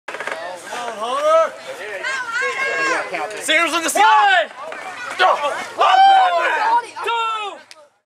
Signal Kick From HDP To Kraze
Signal Kick from TEW Champion Hunter Dude Preston to Anthony Kraze from September 23, 2012 at Vicious Circles. TEWrestling.vze.com